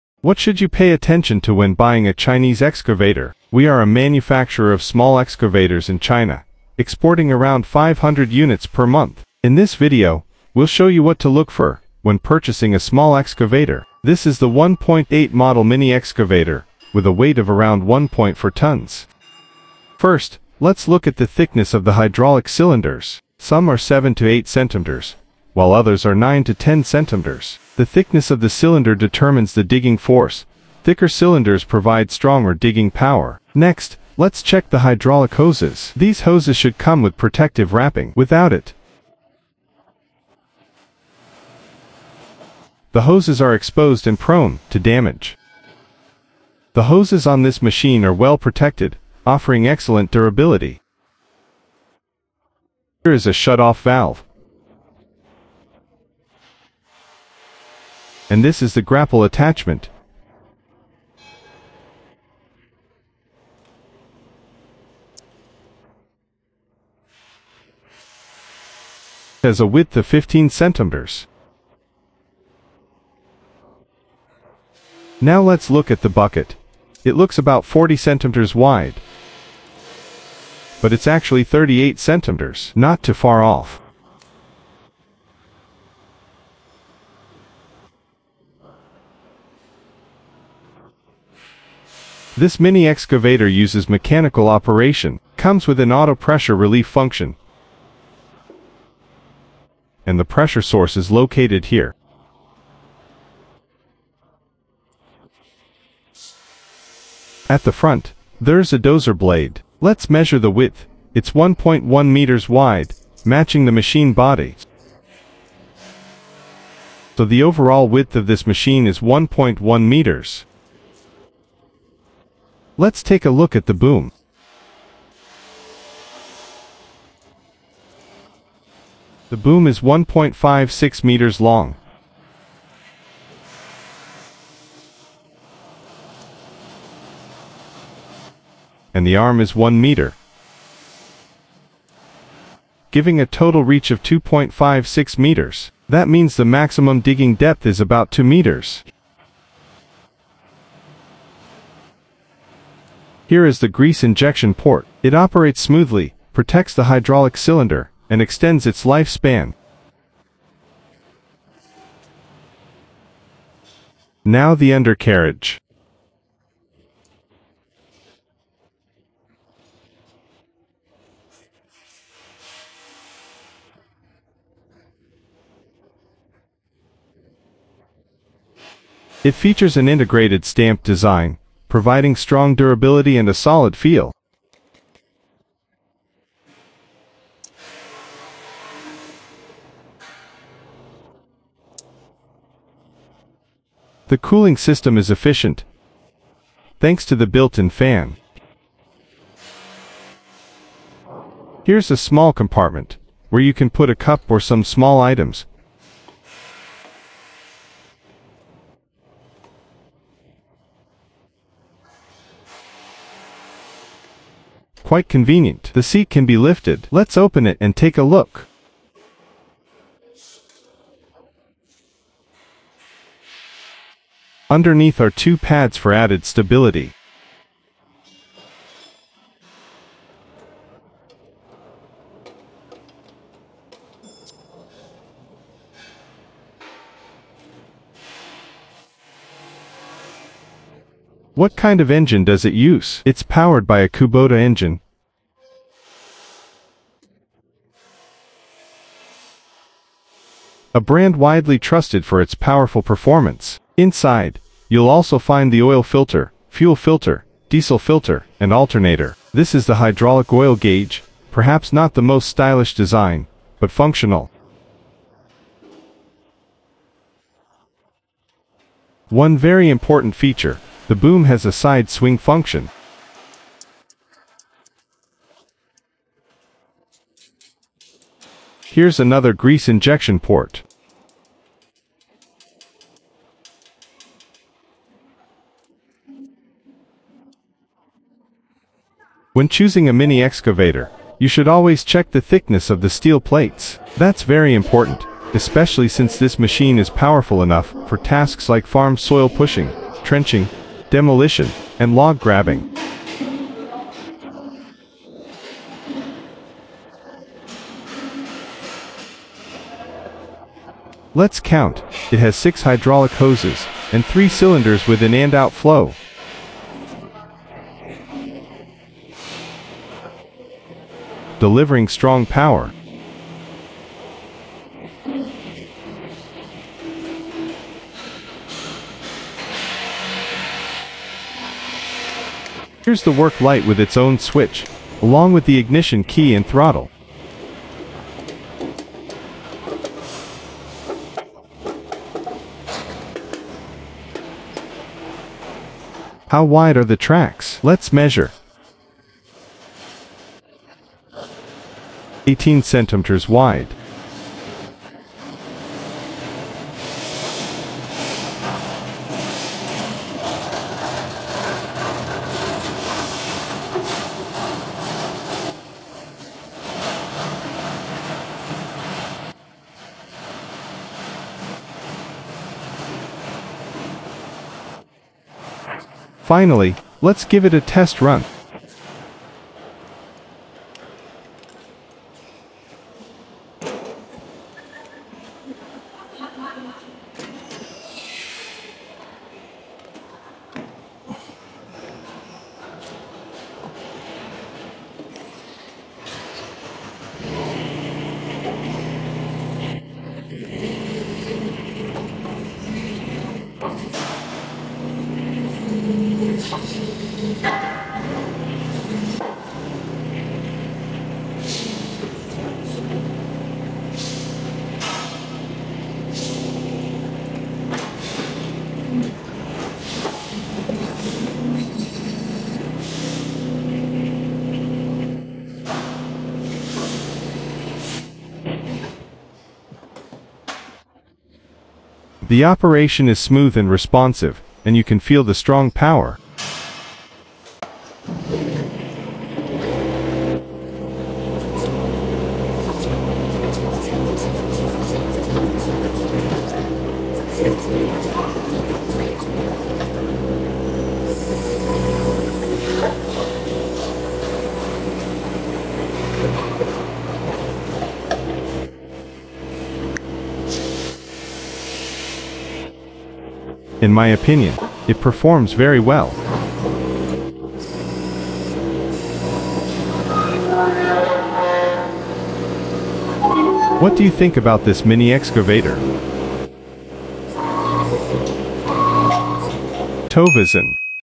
Top 10 Chinese Mini Excavator Brands: Why TOVISEN 18 Stands Out​
China is home to some of the world’s leading mini excavator manufacturers, with brands like XCMG, SANY, LiuGong, Lonking, and Sunward dominating the market. Among these industry giants, TOVISEN has carved out a reputation for producing robust, high-performance compact excavators—and the TOVISEN 18 model is a perfect example of engineering excellence.

Designed for versatility and durability, the TOVISEN 18 features intuitive pilot-operated controls, providing smooth and precise handling even in the most confined spaces. Its reinforced body ensures long-term reliability under demanding working conditions. What truly sets this model apart is its compatibility with the Kubota 3-cylinder water-cooled engine, known for its fuel efficiency and low emissions.

Additionally, the internally routed hydraulic hoses along the arm are a smart design detail, protecting them from external damage and reducing maintenance costs. Whether for construction, landscaping, or urban projects, the TOVISEN 18 combines power, agility, and innovation in a compact form.

For those seeking a dependable and agile mini excavator, the TOVISEN 18 represents both top-tier Chinese manufacturing and practical ingenuity.

Explore the difference today—where strength meets precision.